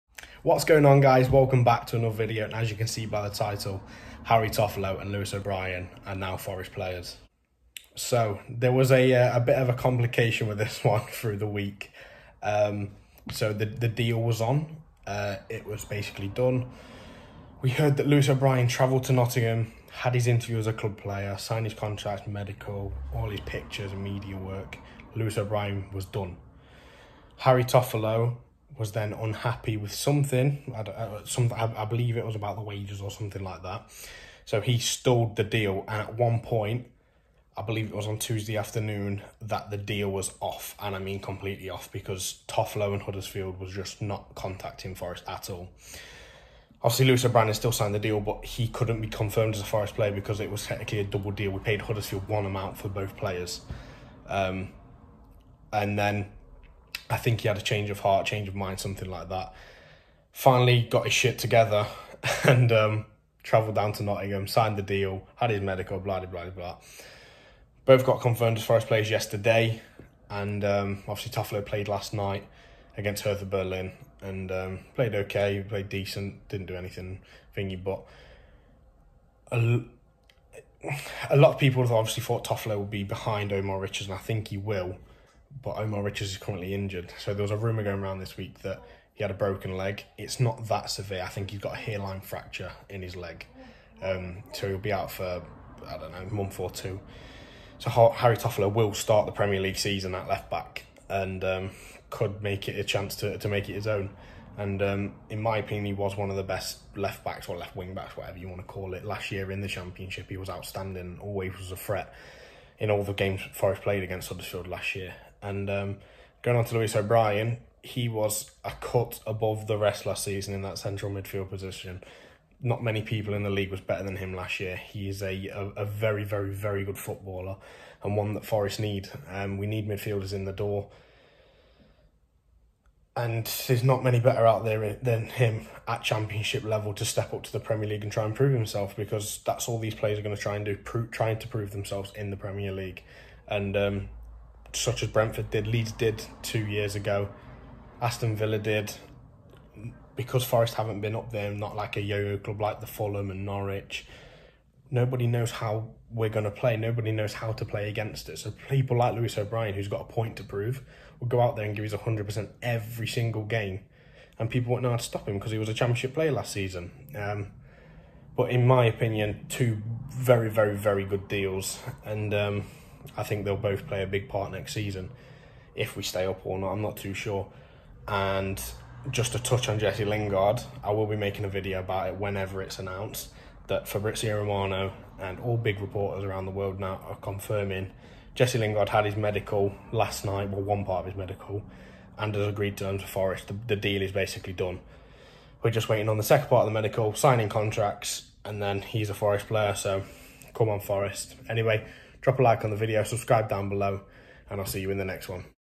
FOREST SIGN TOFFOLO AND O’BRIEN!?
My thoughts on new Forest duo Harry Toffolo and Lewis O’Brien!